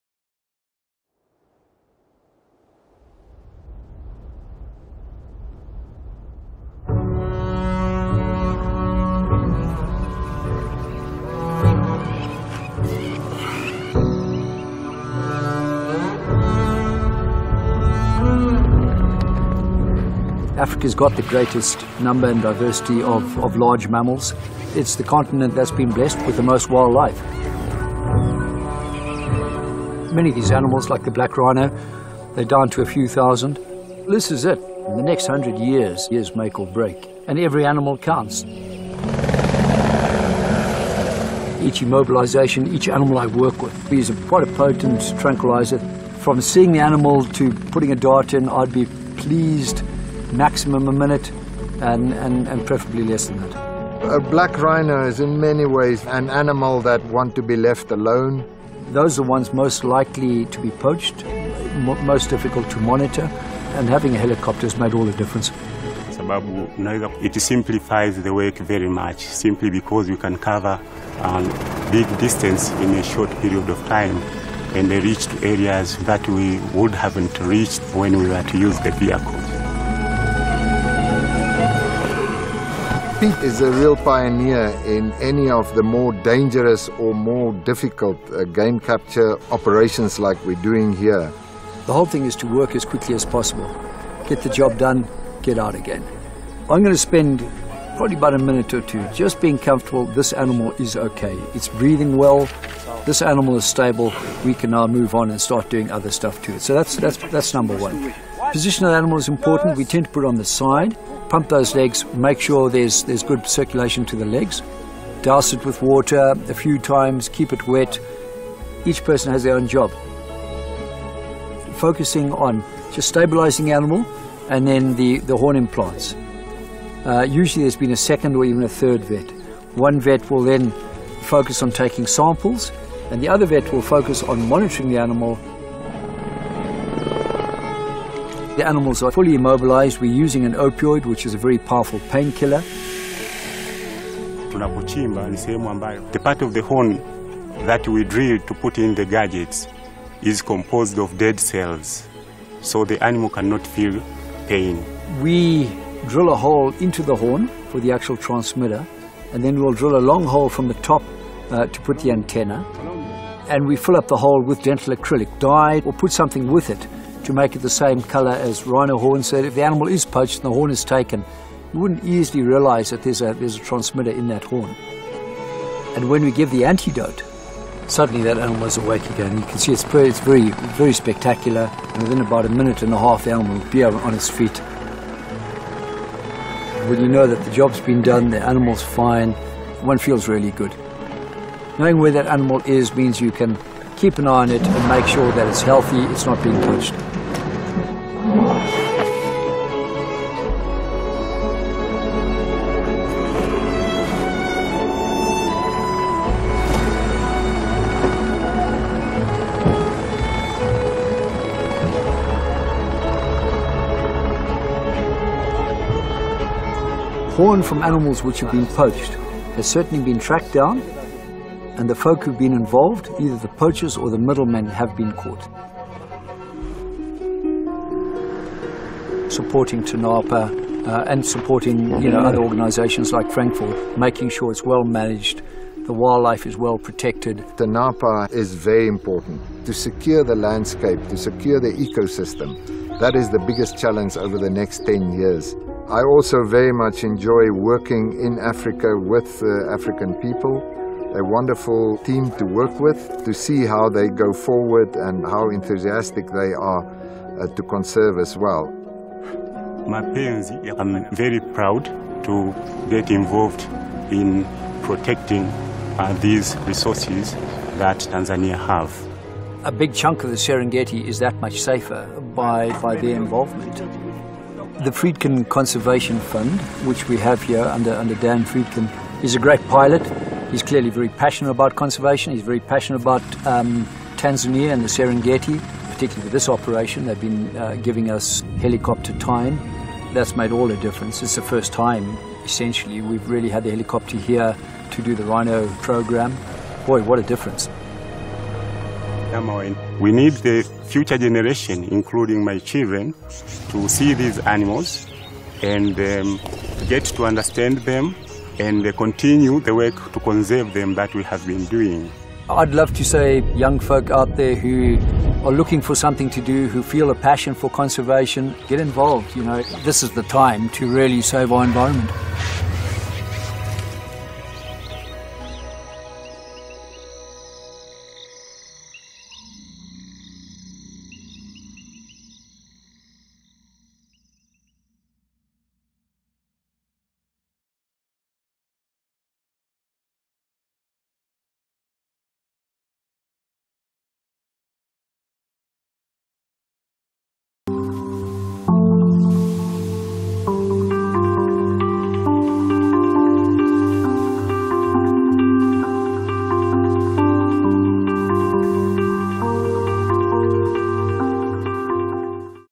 See What It Takes to Hide a Secret Tracker in a Rhino Horn | Short Film Showcase
In Tanzania, a team of veterinarians are on a covert mission to protect the black rhinoceros from poaching.

The operation begins in the air, sedating the animal with a tranquilizer dart sent by helicopter. Once the ground team moves in safely, their first objective is to make sure the creature is stable before carefully drilling a hole into the back of its horn. This area of a rhinos' horn is comprised of dead cells, which means the animal will not feel any pain during the procedure. A radio transmitter is then inserted into the opening, and dental acrylic and paint are used to conceal the device so it matches the rest of the horn. After the operation is complete, this newly-installed beacon will send a signal with the animal's location back to the team every minute. In addition to monitoring the rhinos' movements, the team can also track down any poachers with a tagged horn in their possession.

In this exciting film by Pursuit Aviation, watch the action unfold as the Friedkin Conservation Fund teams up with Tanzania National Parks, TAWA, and the Frankfurt Zoological Society to place beacons in 23 rhinos.